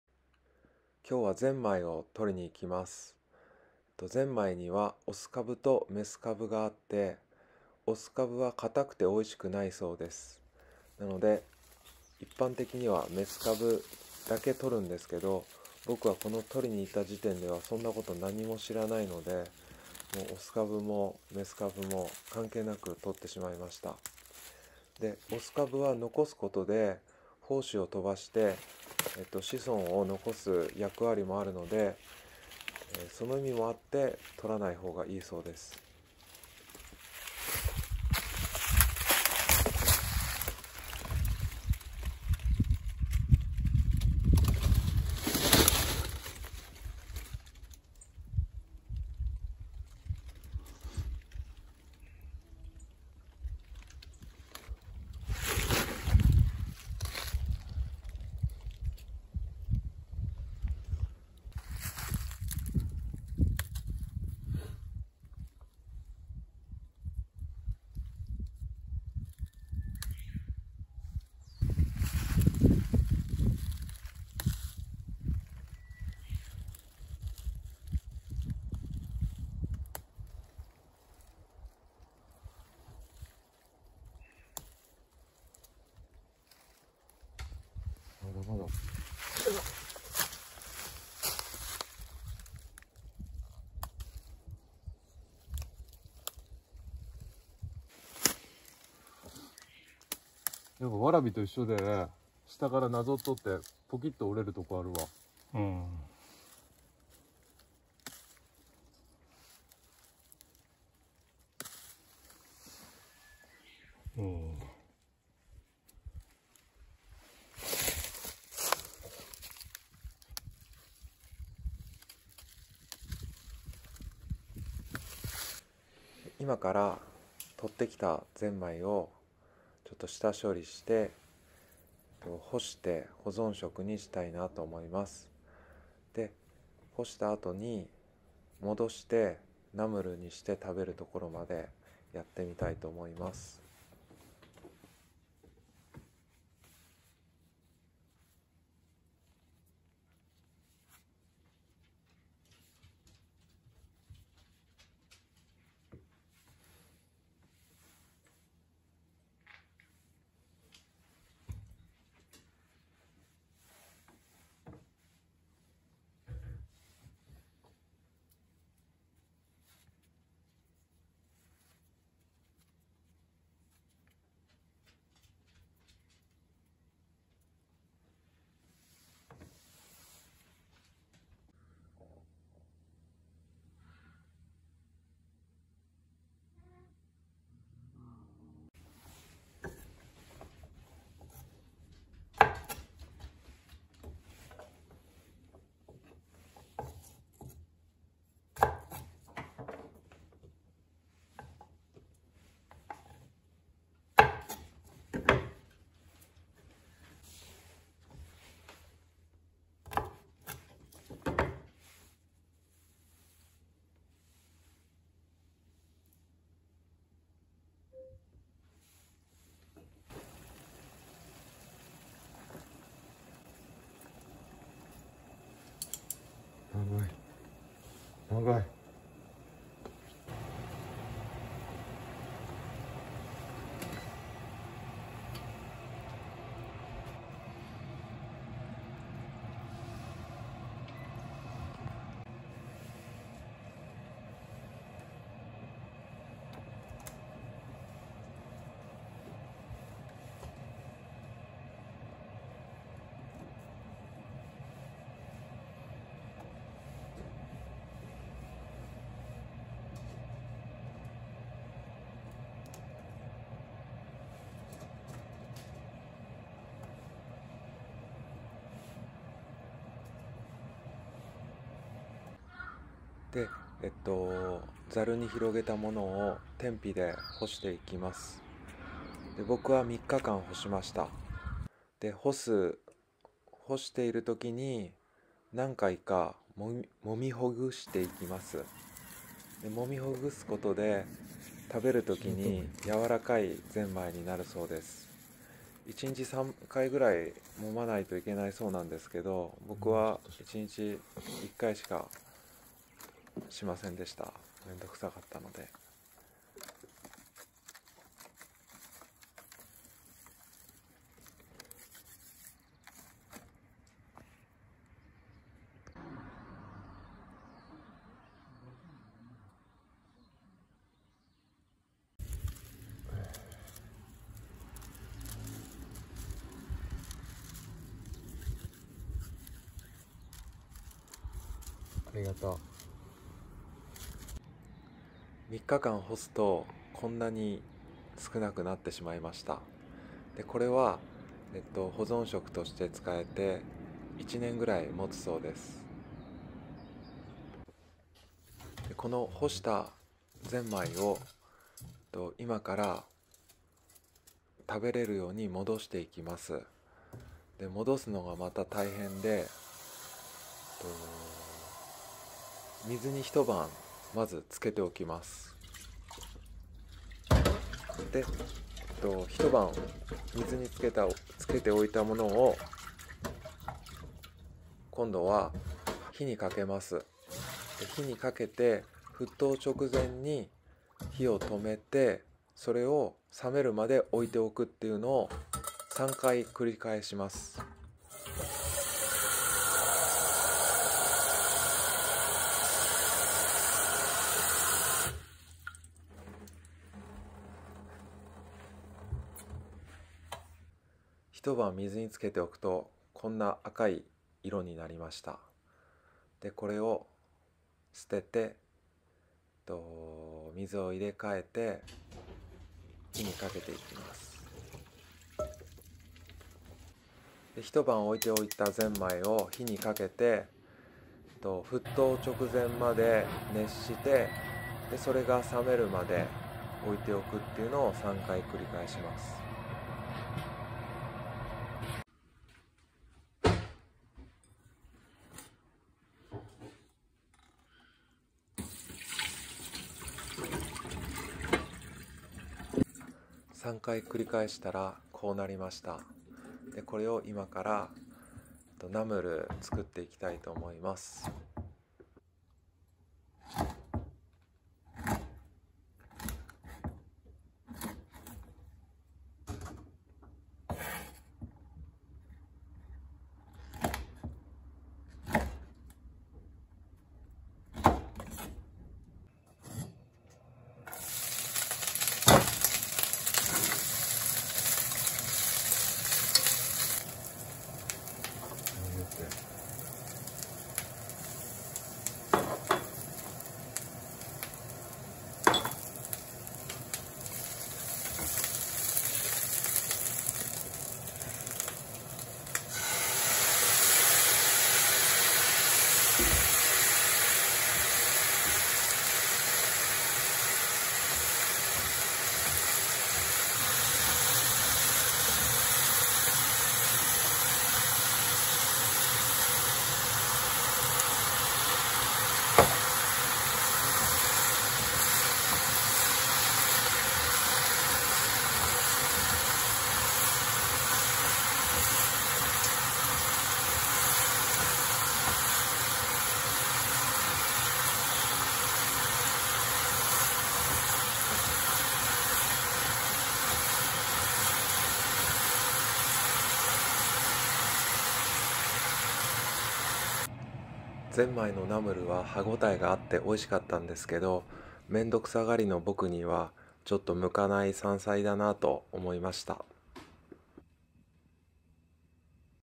（山菜採り編②）ぜんまいを採って、干して戻してナムル
初めて、ぜんまいを収穫し、干し、戻し、料理しました。
面倒くさがりな僕には、
干すのに３日。
戻すのに２日。
かかるぜんまい料理は向いていないと、改めて実感しました。